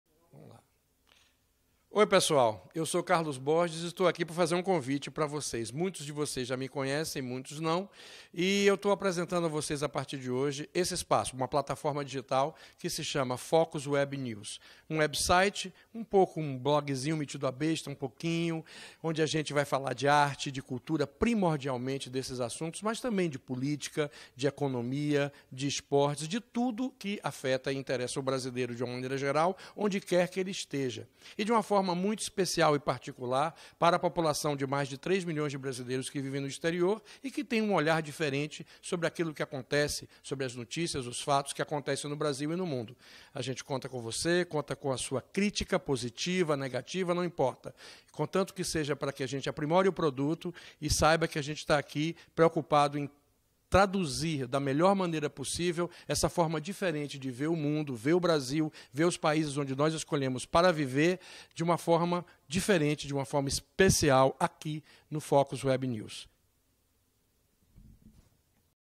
Carlos Borges Apresenta Focus Web News
O jornalista Carlos Borges apresenta a plataforma de mídia digital Focus Web News, focada em artes, cultura, política, economia e esportes, e primordialmente destinada aos mais de 3 milhões de brasileiros que residem no exterior.